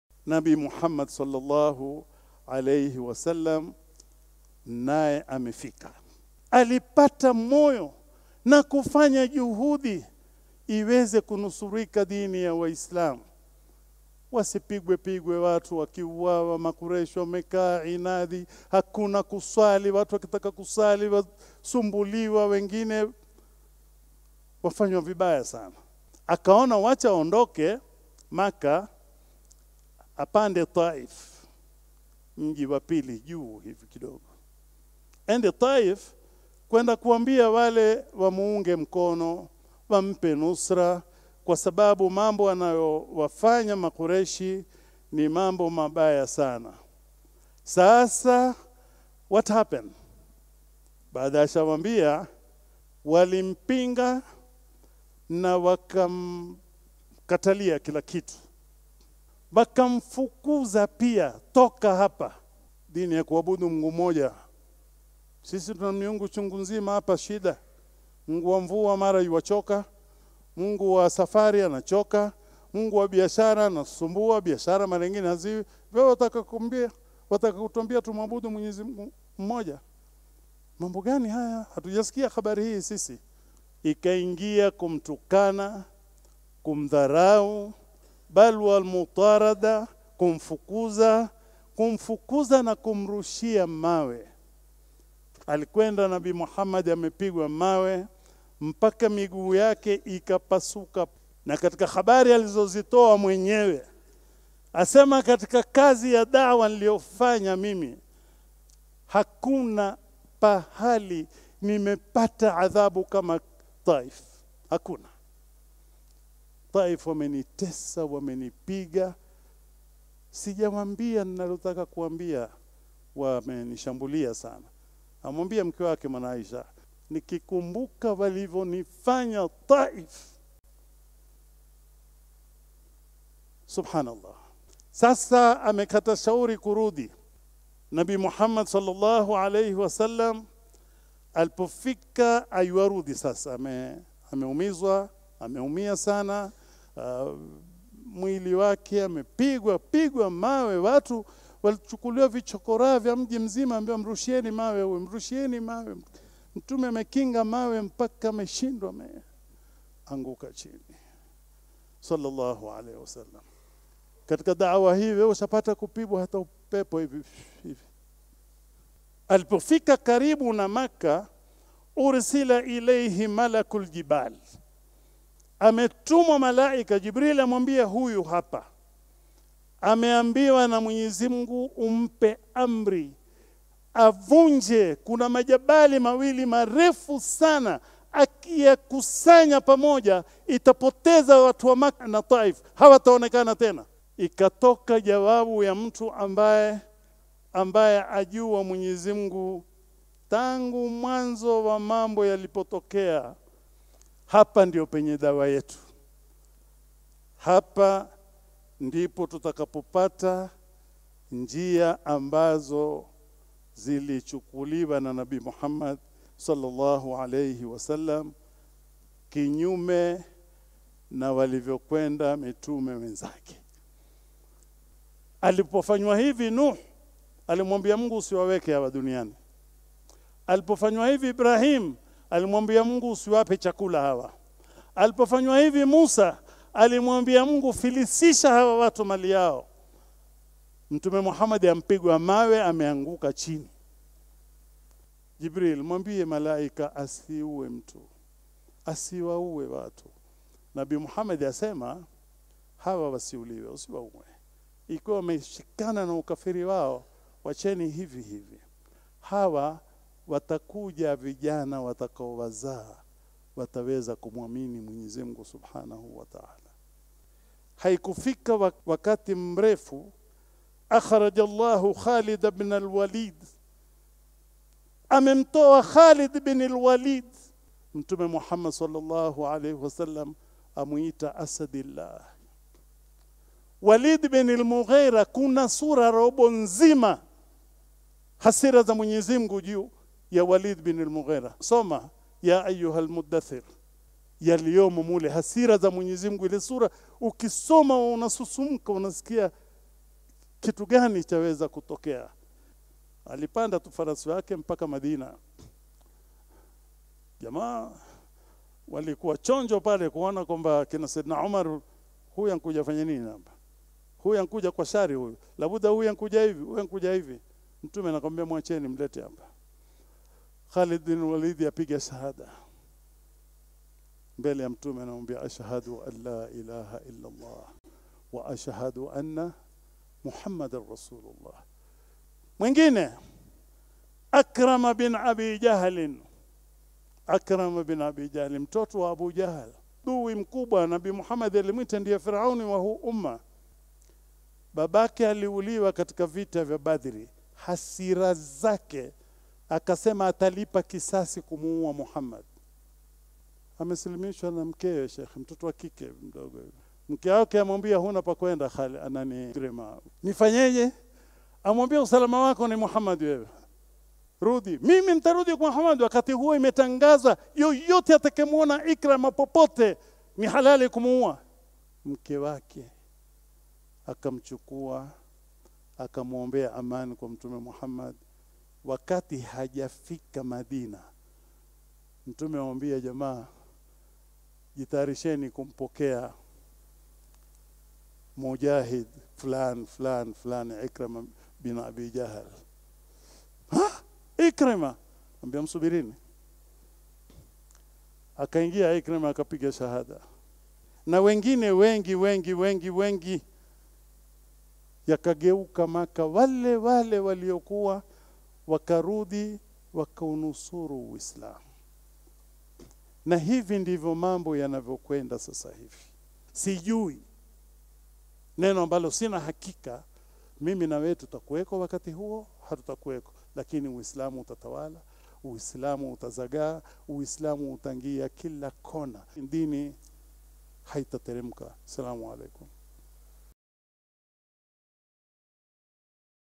Mtumeﷺ Alivyoteswa Mji wa Taif Na Kusilimu Kwa Wana Wa Adui Zake | Sheikh Juma Amir
Horizon TV | Beacon For The Nation